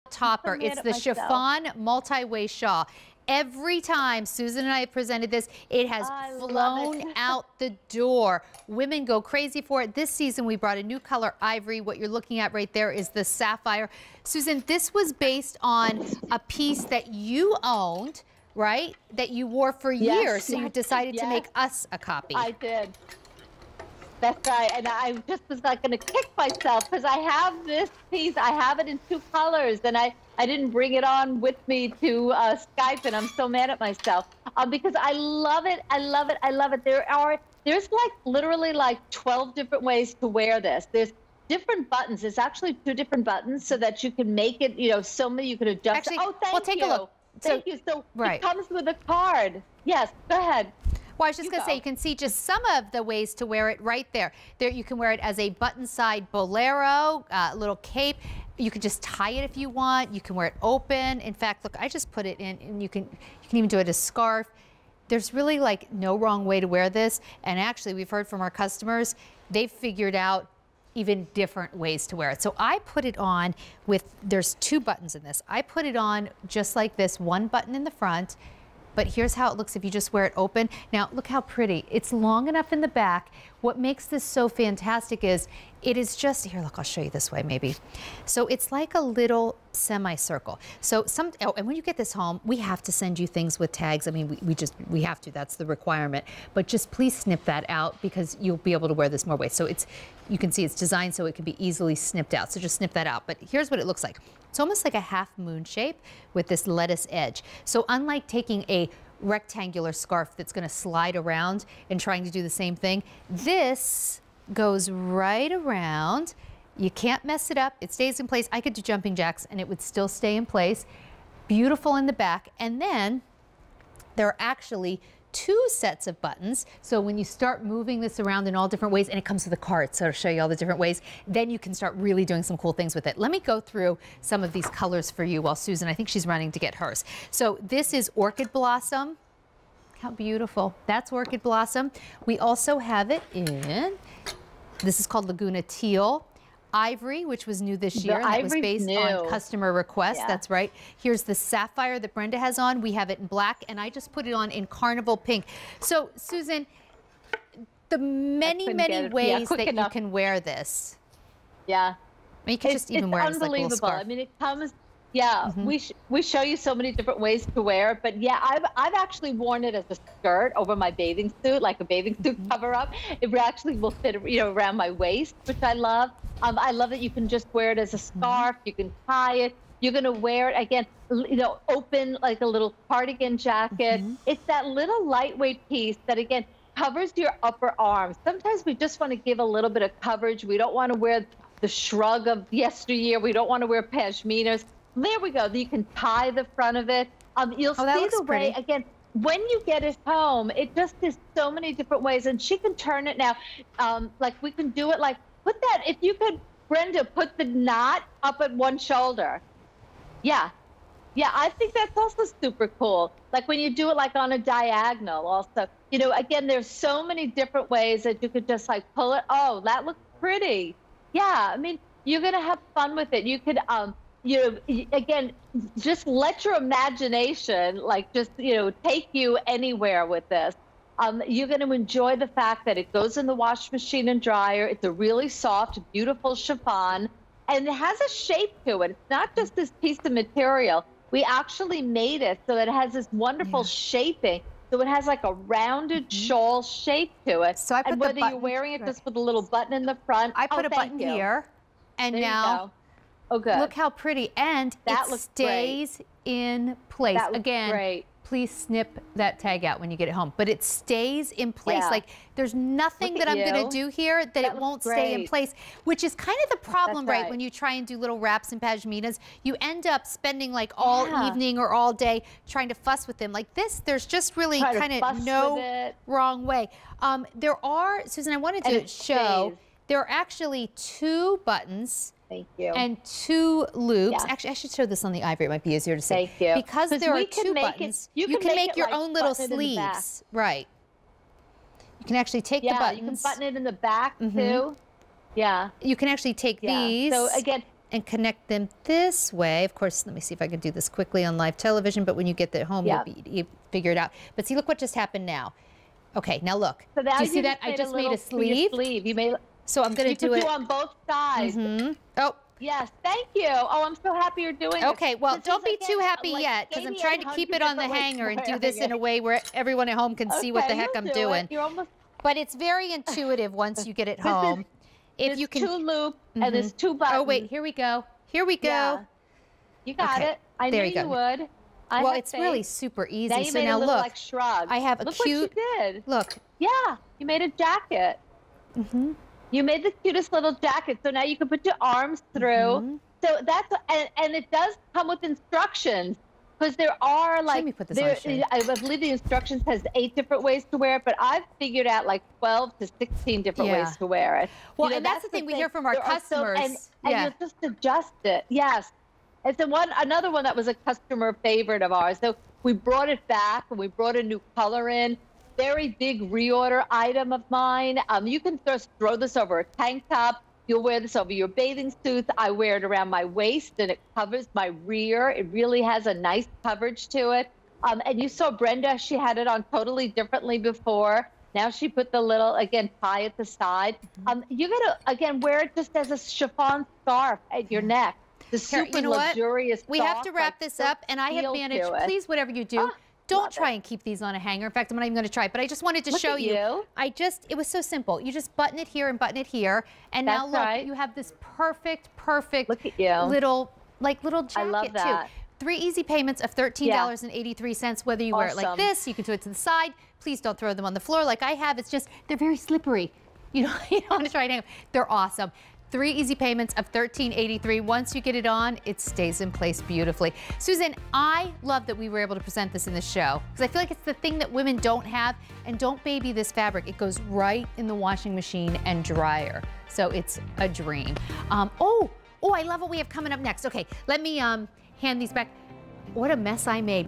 Susan Graver Chiffon Multi-Way Shawl on QVC
Susan Graver Chiffon Multi-Way Shawl

This chiffon shawl can be worn a number of ways, so whether you team it with a tank top and jeans or a sheath dress, the end result is a fabulous ensemble. From Susan Graver.